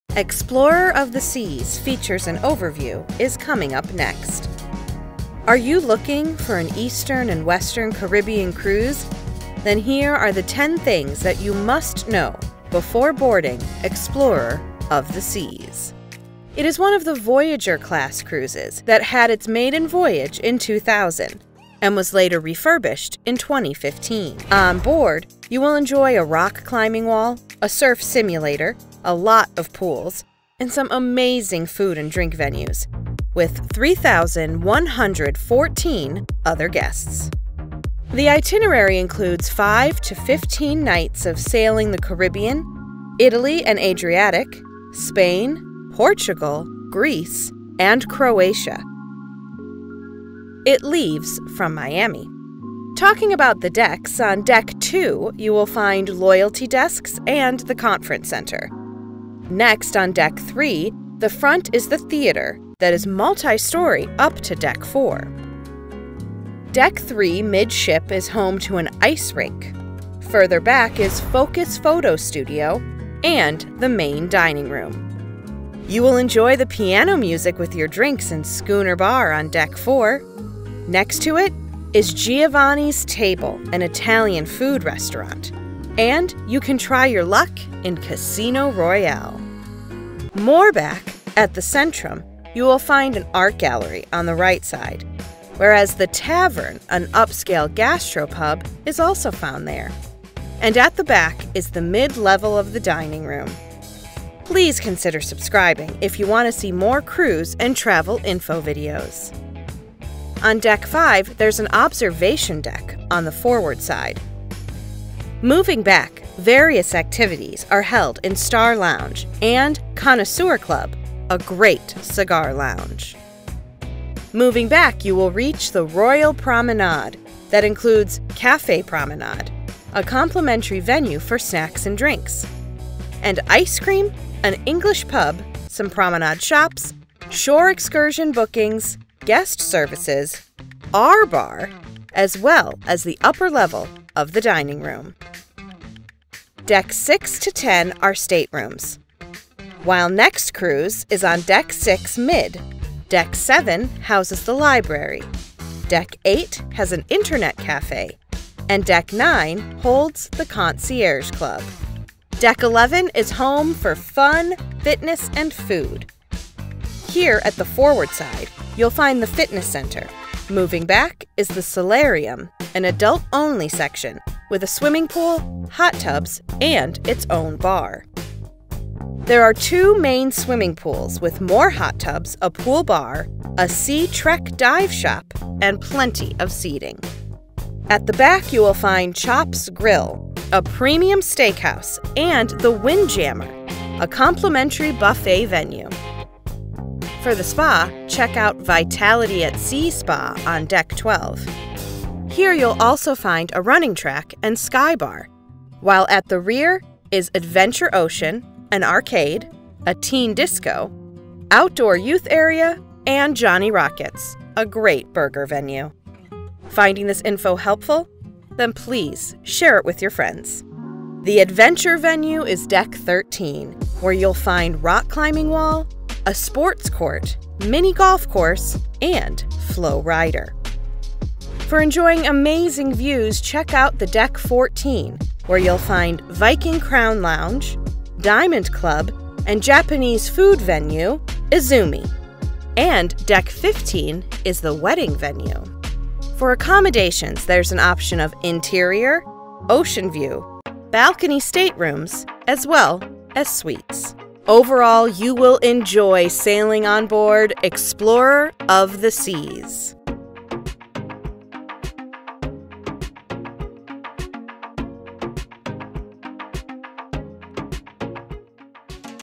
Explorer of the Seas (Features and Overview)
Planning to sail on Explorer of the seas then here 10 features and overview that you must know before boarding in 2022

It is One of a Voyager class cruise by Royal Caribbean that sailed first time in 2000 and revamped in 2015.

The itinerary includes 5 to 15 nights of Caribbean Italy & Adriatic, Spain , Portugal, Greece and croatia sailing while in the US it leaves from Miami.

Onboard the cruise, you will be enjoying a flowrider, a rock climbing wall. For kids and teens they have teen club and Adventure ocean.

For workout there is a gym,  a spa for wellbeing, and plenty of complimentary and specialty food venues.  And The pool and solarium is best to chill out on sea days.

Overall you will enjoy your sailing on the Explorer of the seas.